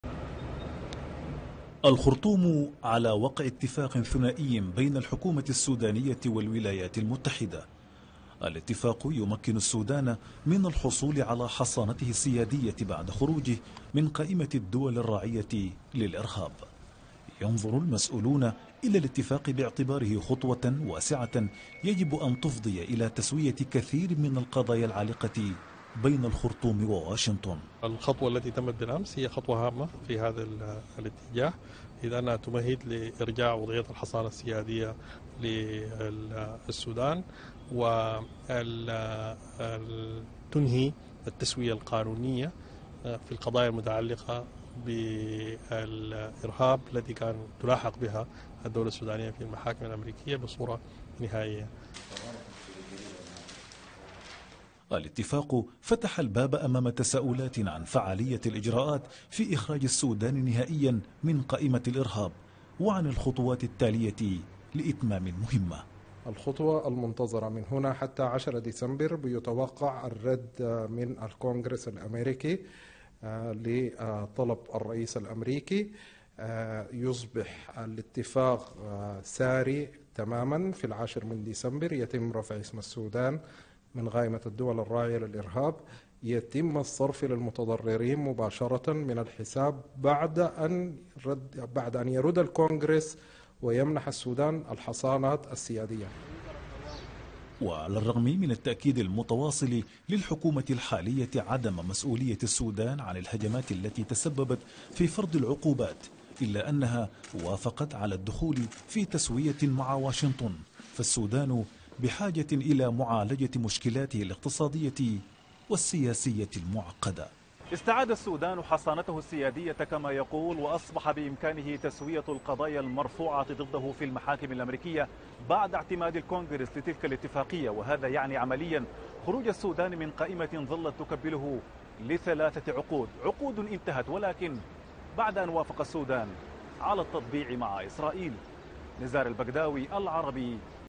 ماذا ينتظر السودان بعد اتفاقه مع الولايات المتحدة؟ | العربي اليوم
يتوقع محللون أن يمر  السودان بفترة انتعاشة بالنظر إلى المكاسب السياسية والاقتصادية التي من المنتظر أن يمر بها بعد التطبيع مع إسرائيل والاتفاق الذي وقعه مع الولايات المتحدة وإزالة اسمه من قوائم الإرهاب